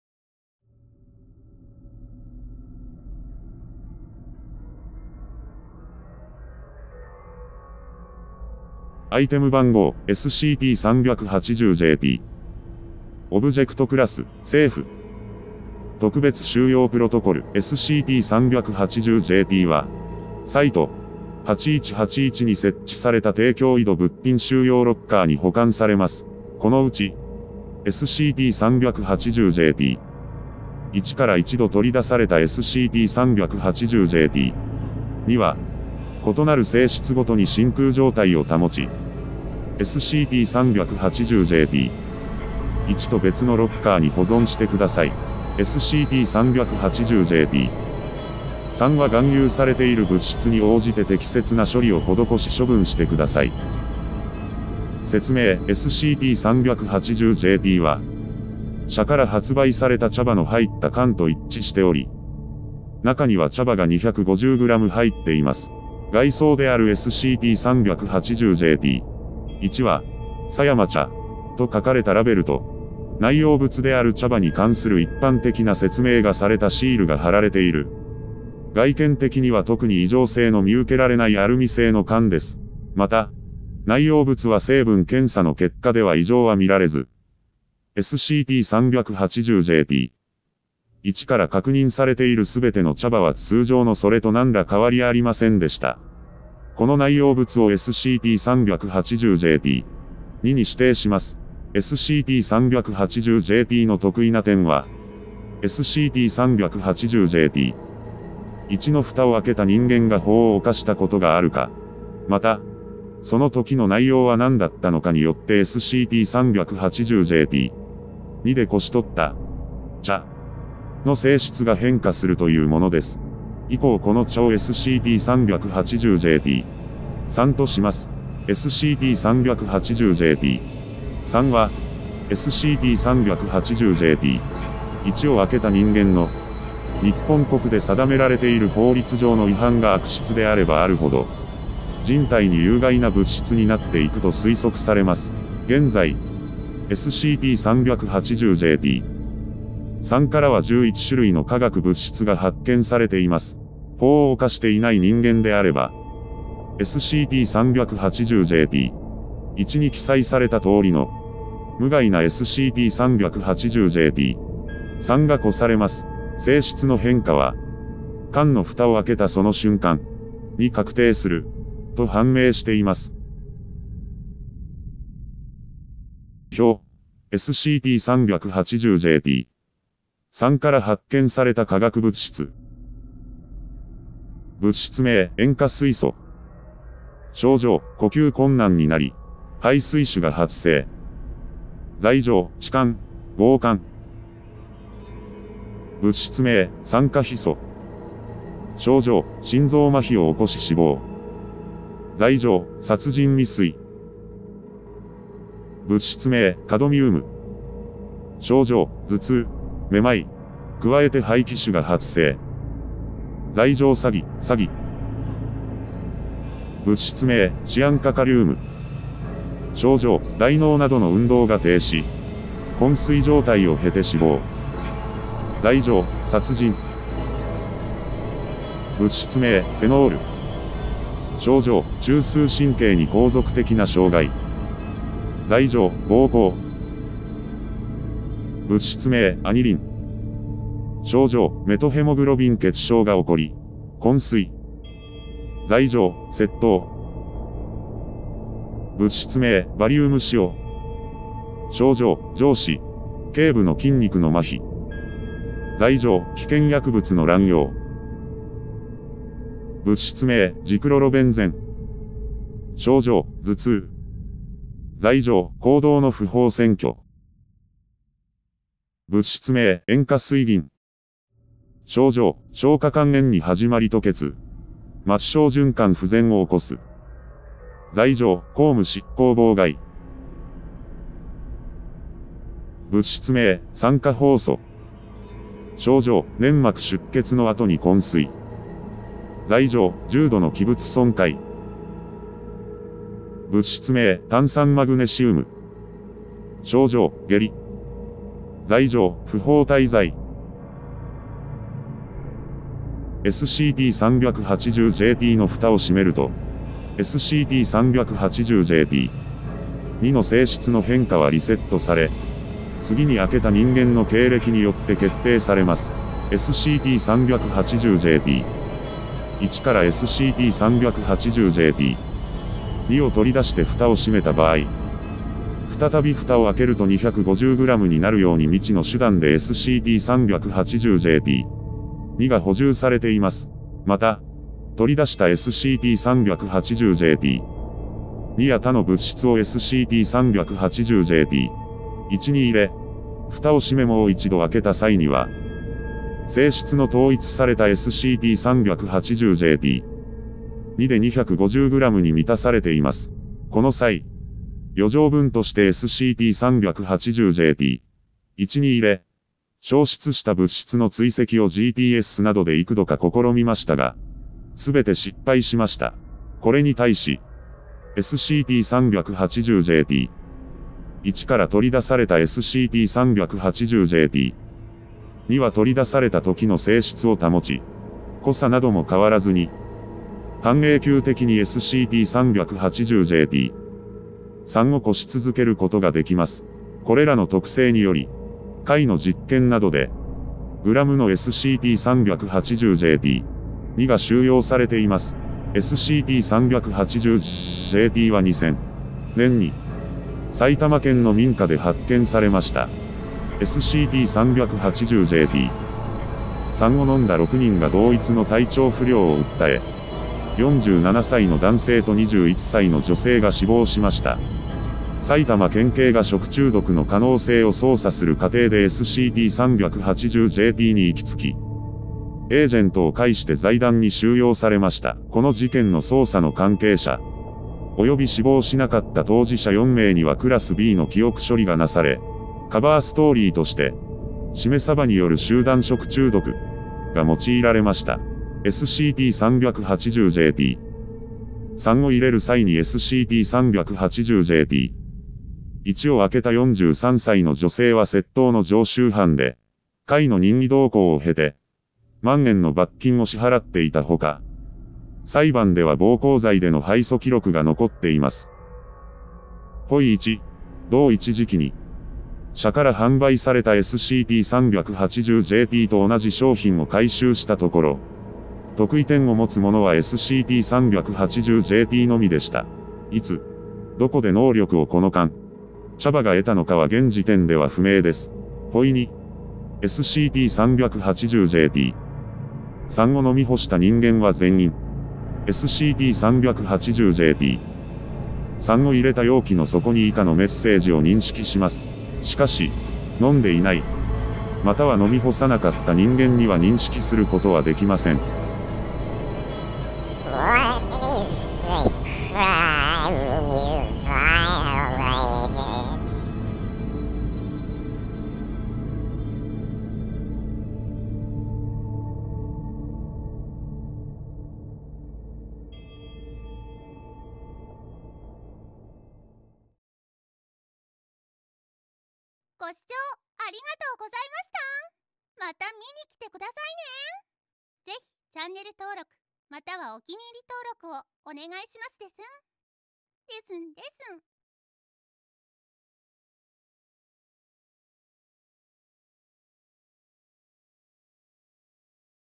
SCP財団機密データ：SCP-380-JP - 狭山茶
オブジェクトクラス: Safe

特別収容プロトコル: SCP-380-JPは、サイト-8181に設置された低脅威度物品収容ロッカーに保管されます。